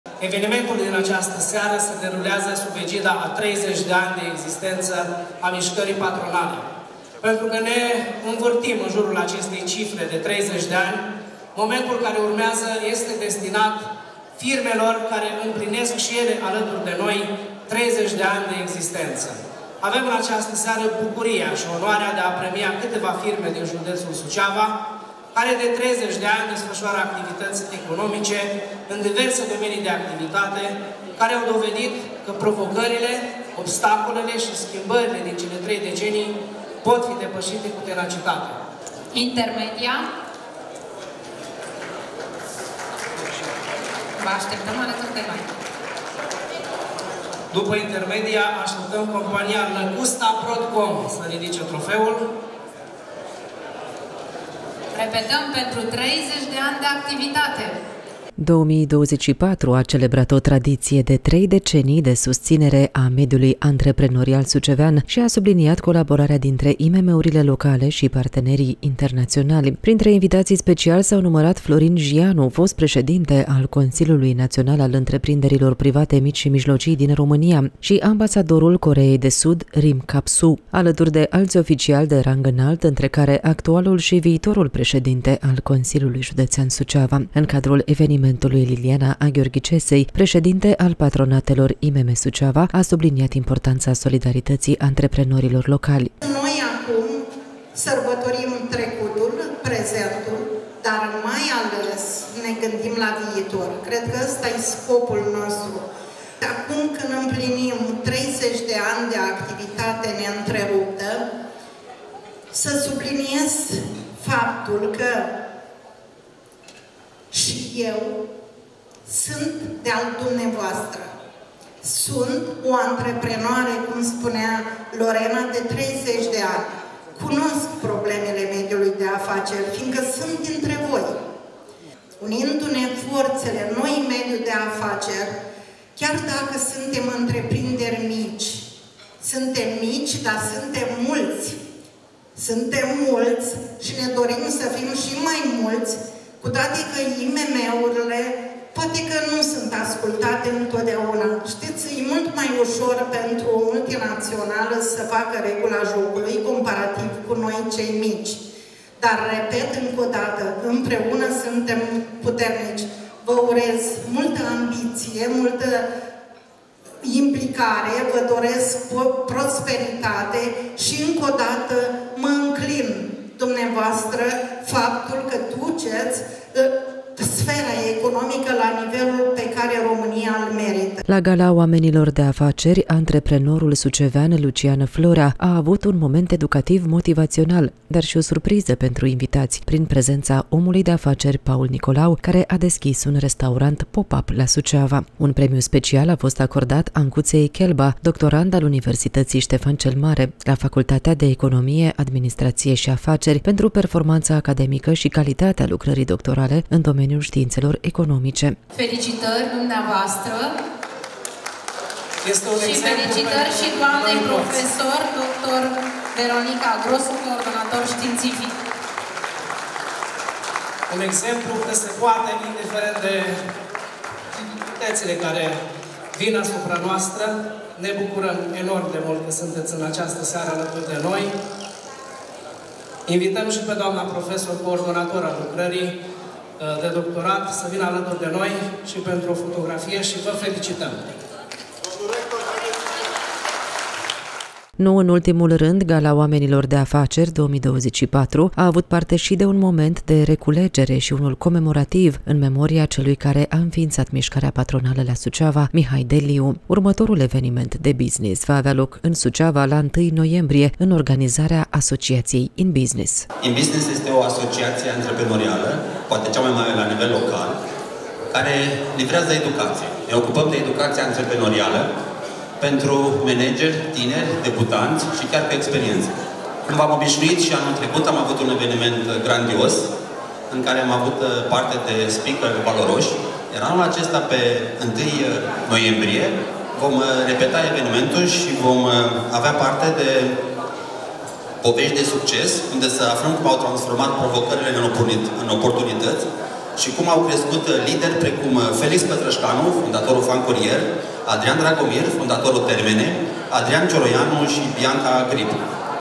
Inter-Media SRL, premiu pentru 30 de ani la Gala Oamenilor de Afaceri 2024
GALA OAMENILOR DE AFACERI – ediția 2024 a marcat 30 de ani de la înființarea primei organizații patronale IMM din județul Suceava. Evenimentul a reunit antreprenori locali, reprezentanți ai autorităților și instituțiilor publice, precum și parteneri din țară și străinătate. Au fost acordate premii de excelență pentru firmele cu rezultate remarcabile în 2023, dar și pentru companiile sucevene cu o activitate de peste 30 de ani. Între acestea, și Inter-Media SRL.